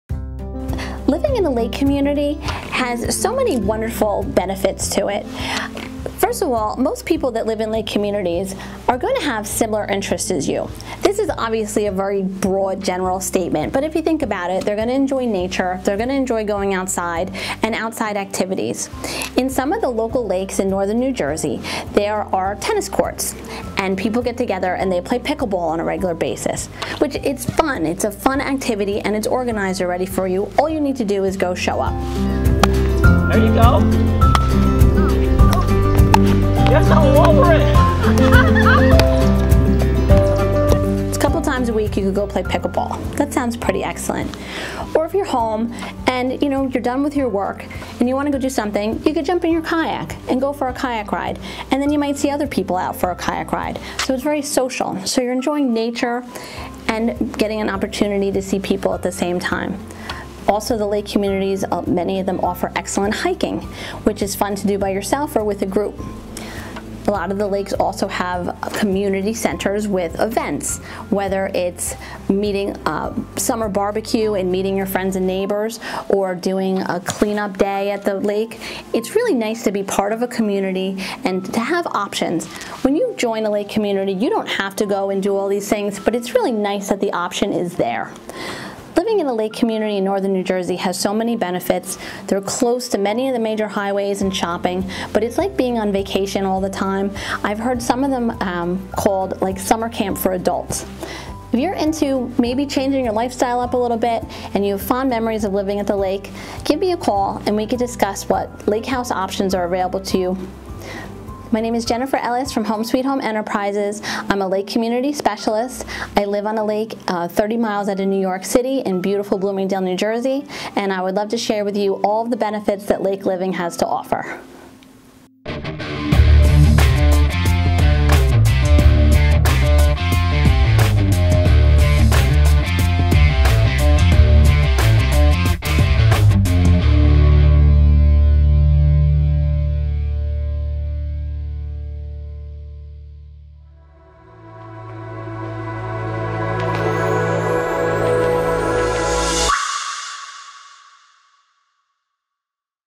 Better to Wake at Glenwild Lake!
Looking for a luxurious lake house? Look no further than Glenwild Lake! With its pristine waters and beautiful surroundings, Glenwild is the perfect place to relax and unwind.